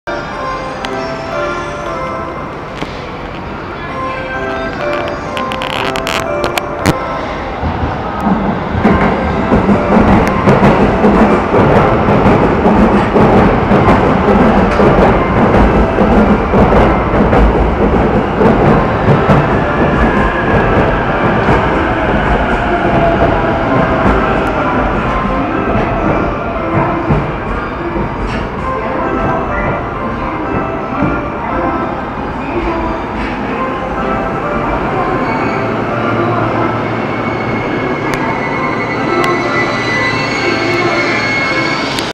JR東京駅下り9番線に、上野東京ライン東海道線直通普通品川行きE531系1238MK410水カツ+E531系K482水カツが到着！
JR東京駅下り9番線ホームJR上野駅側から撮影。

都区内パスを購入しての撮影。

JR上野駅を発車して走行中の、上り15(10+5)両編成上野東京ライン東海道線直通普通品川行きE531系1238MK410水カツ+E531系K482水カツが、JR東京駅下り9番線ホームに到着。